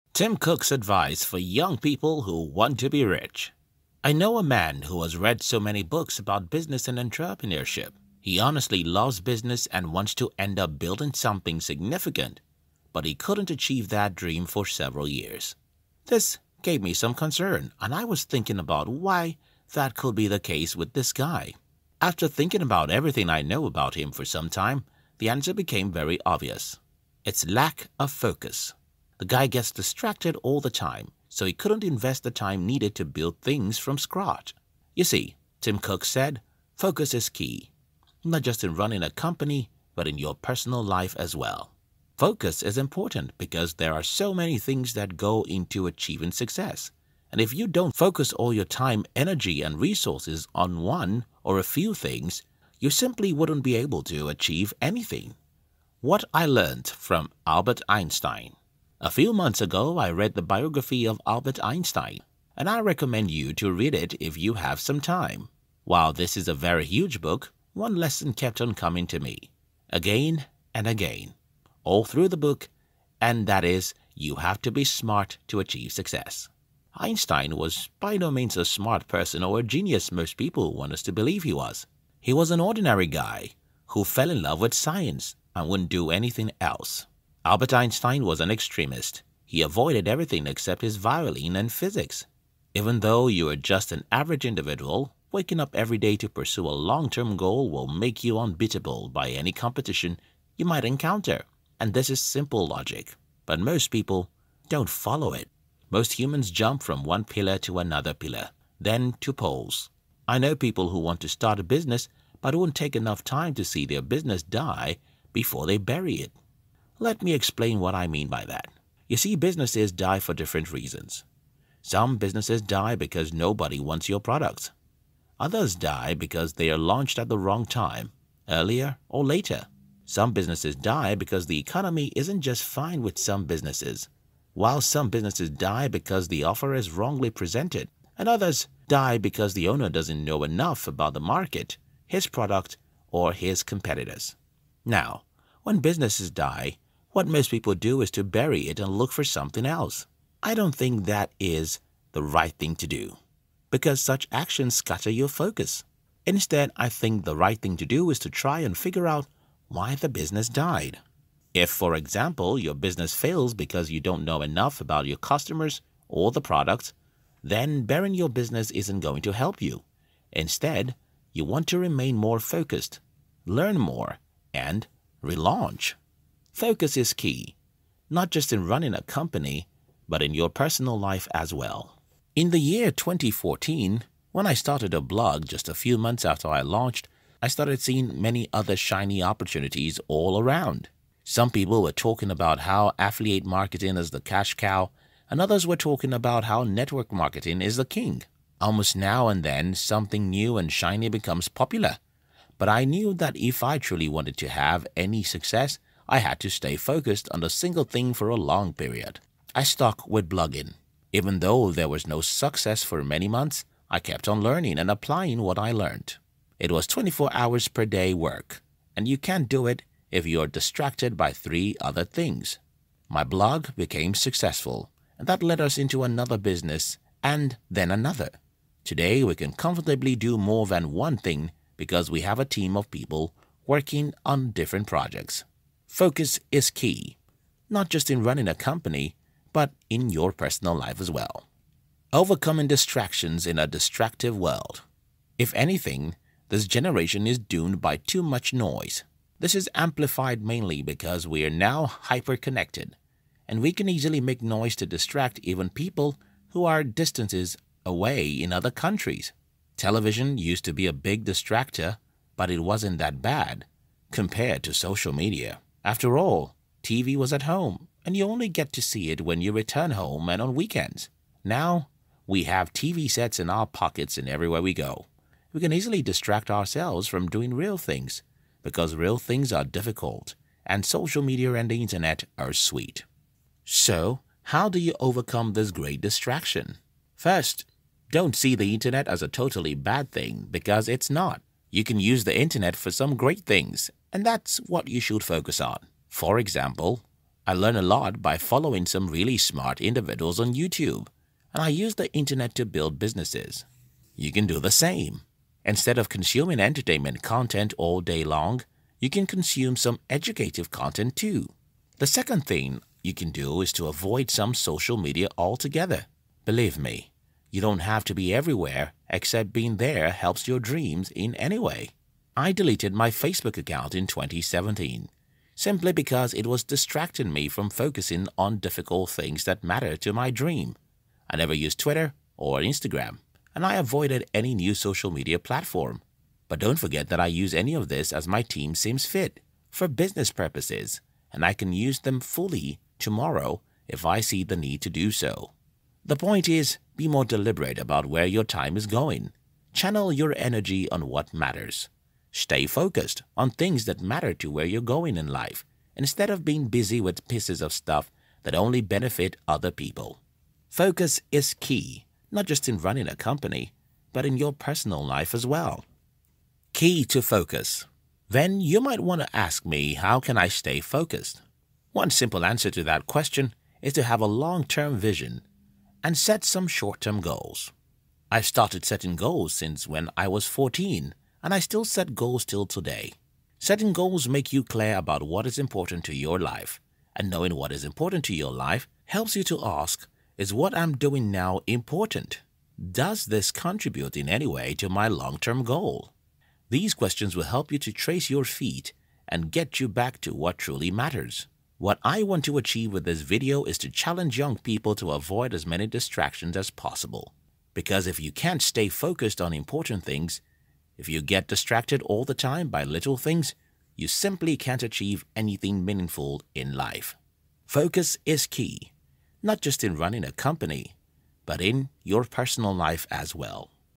Tim Cook's advice, for young people who want to be Rich
I know a man who has read so many books about business and entrepreneurship. He honestly loves business and wants to end up building something significant but he couldn't achieve that dream for several years.
This gave me some concern and I was thinking about why that could be the case with this guy. After thinking about everything I know about him for some time, the answer became very obvious; it' lacks focus. This guy gets distracted all the time so he couldn't invest the time needed to build things from scratch.